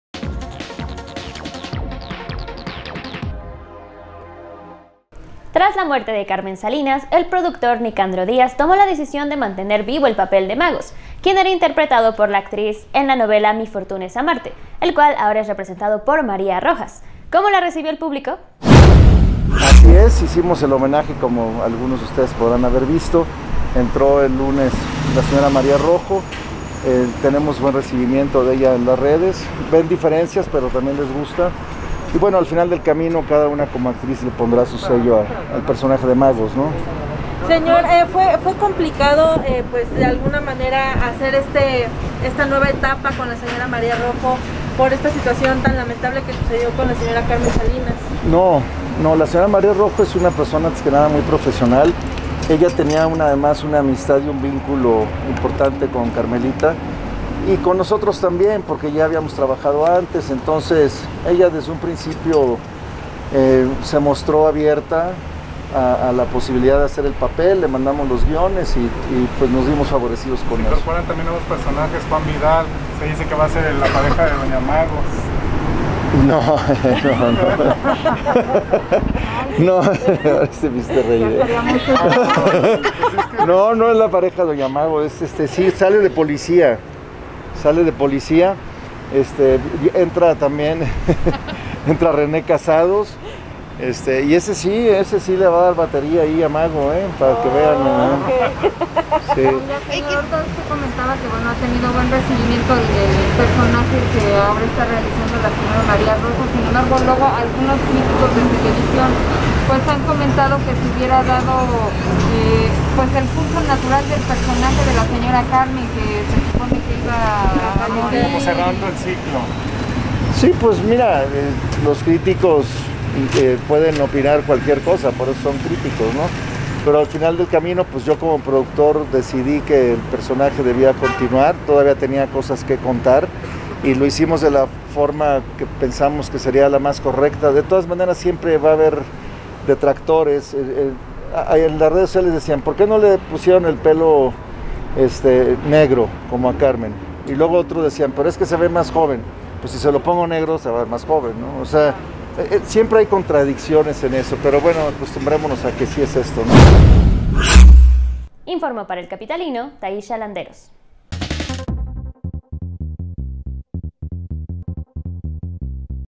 Nicandro Díaz habla sobre María Rojo en el papel de "Magos"
Nicandro Díaz habla sobre María Rojo en el papel de "Magos", interpretado anteriormente por Carmen Salinas.